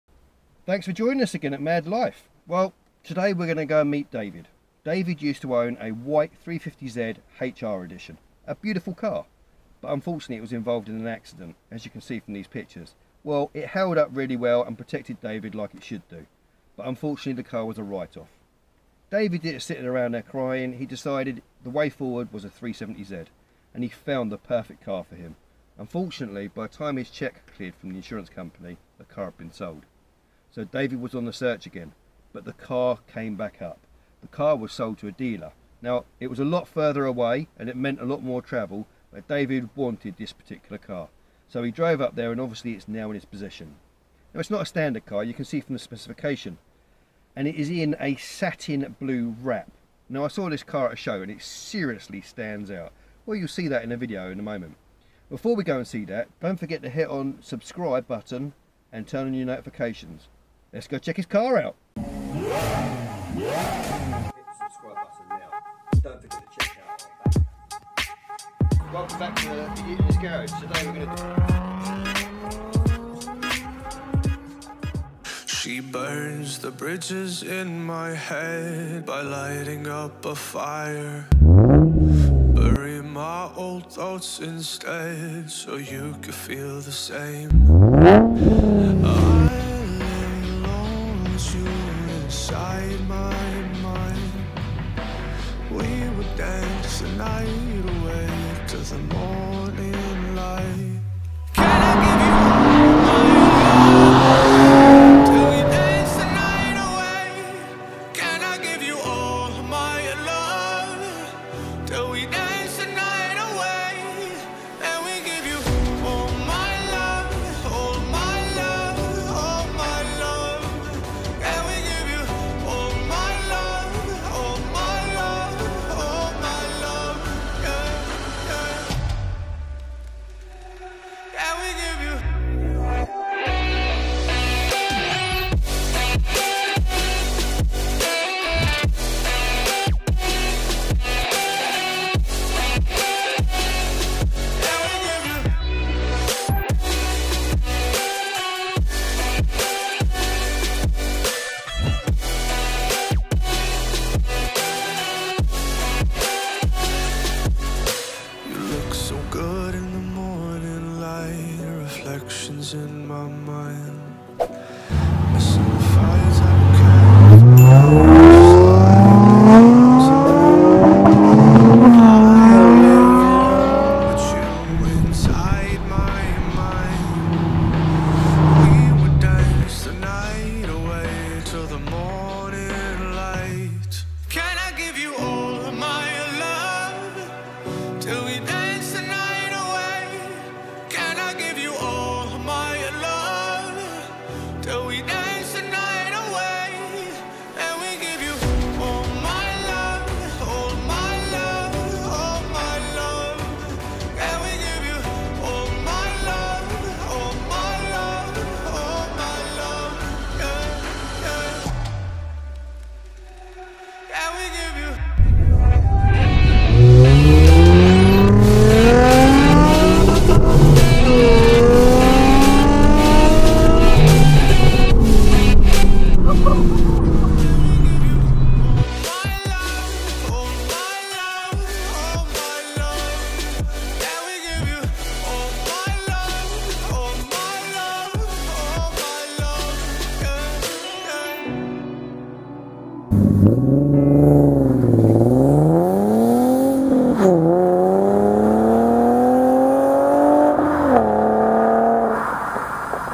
All my Love, Nissan 370z Black edition
David's 370z Black edition stands out so much in the satin blue wrap it wears and if you can't see you you will definitely hear it, if all of that doesn't float your boat look at the interior, it's to die for.

Amazing day with loads of laughs.